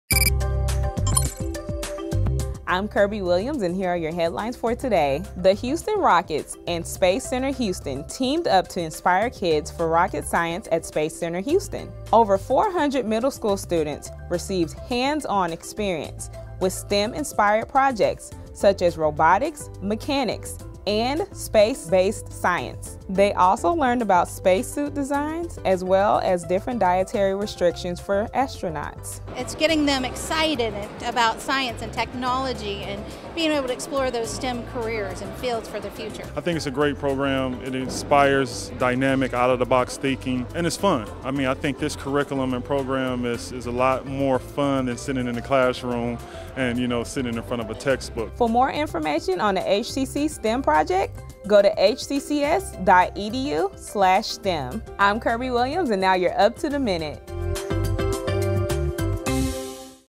Up To The Minute report: March 24, 2016
The Up To The Minute report for Thursday, March 24, 2016, takes a look at the recent HCC Rockets Science workshop at Space Center Houston. A partnership among Houston Community College, the Houston Rockets and NASA, Rockets Science is a program that was created to get students excited about STEM careers. During the workshop, area middle school students had the chance to explore STEM more with some hands-on activities and learn about space. For more information about Rockets Science, please visit hccs.edu/rockets.